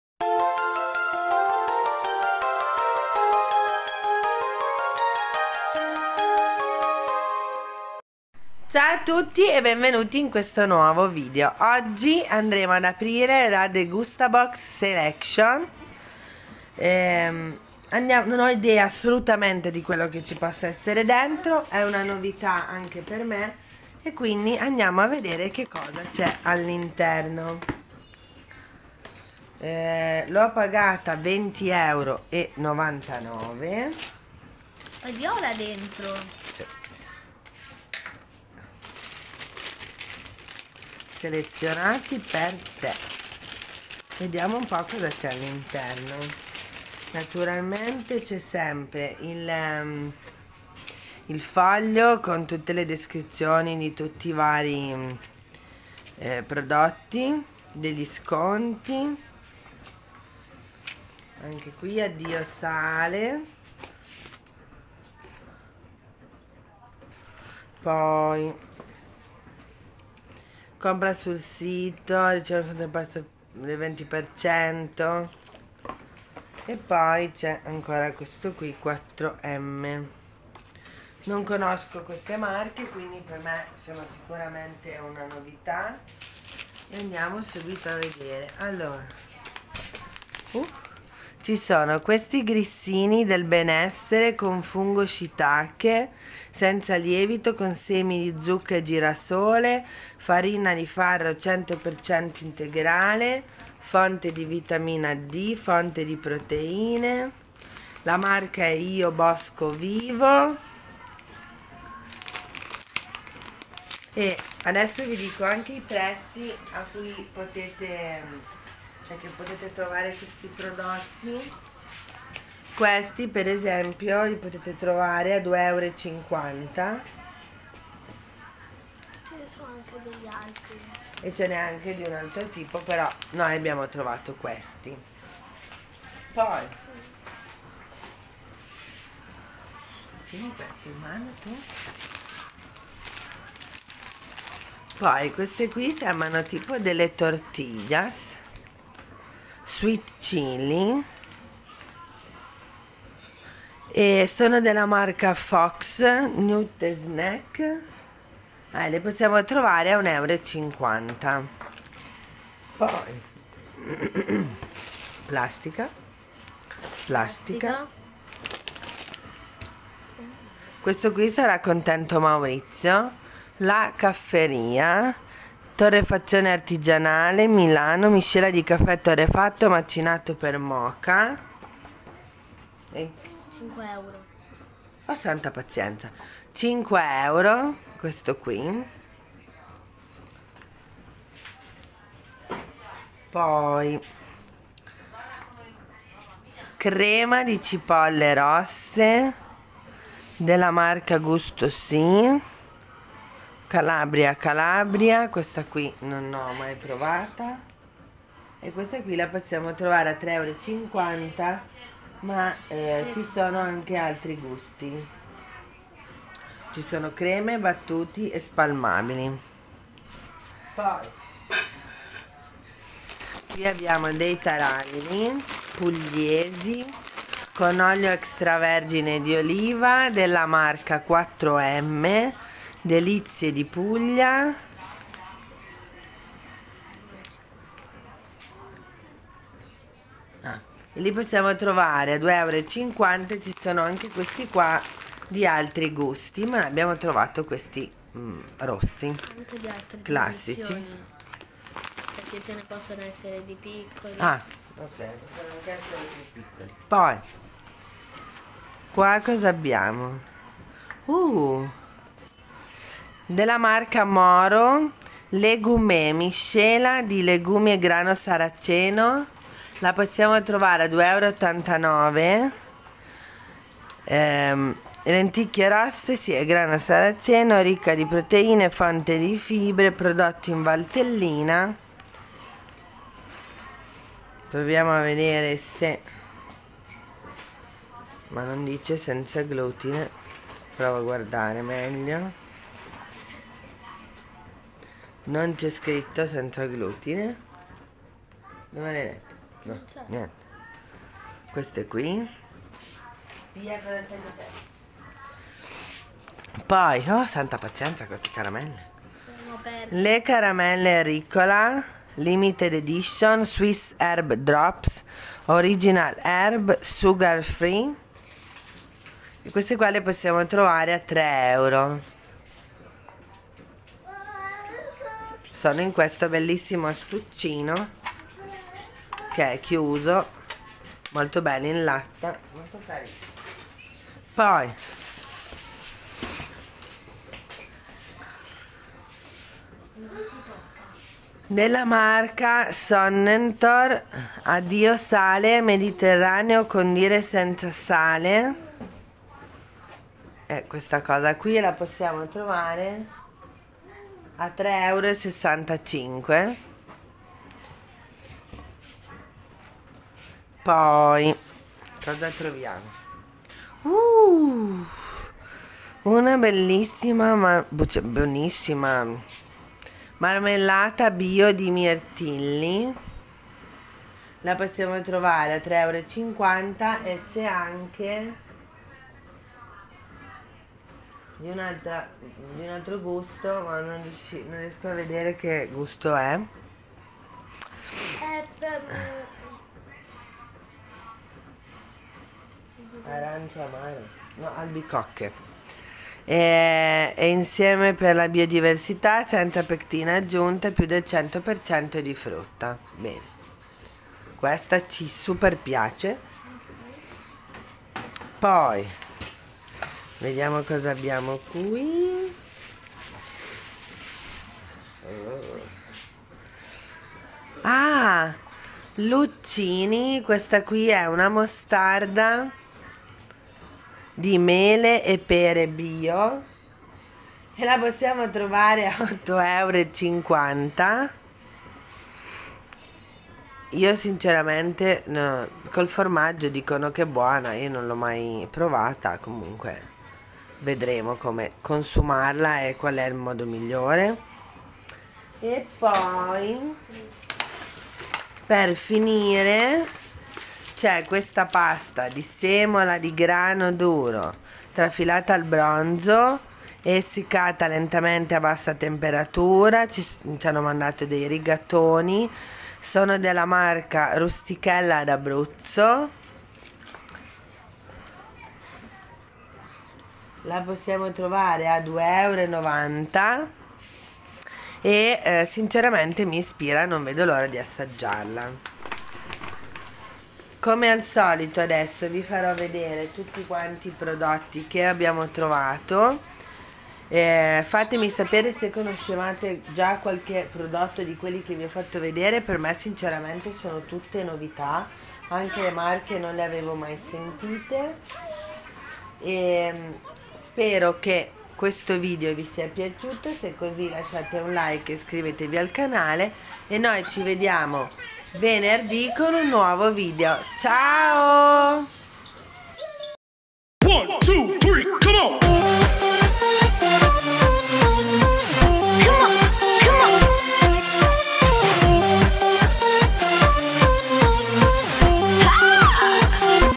📦DEGUSTABOX SELECTION 📦
Ciaoooooooo e benvenuti in questo nuovo video e in questo nuovo format 😊 ho riattivato l'abbonamento Degustabox e ho deciso di condividere con voi quello che trovo 😊 non è una collaborazione ma saranno pagate interamente da noi 😊 vi lascio il link per il sito nel caso foste interessati 😊 fatemi sapere se vi piace 😊

◇♡Con cosa registro ed edito i video:
•Samsung A7, Huawei Mate 20 Lite e Huawei P9 Lite
•Kine Master, My Movie, Linu e YouCut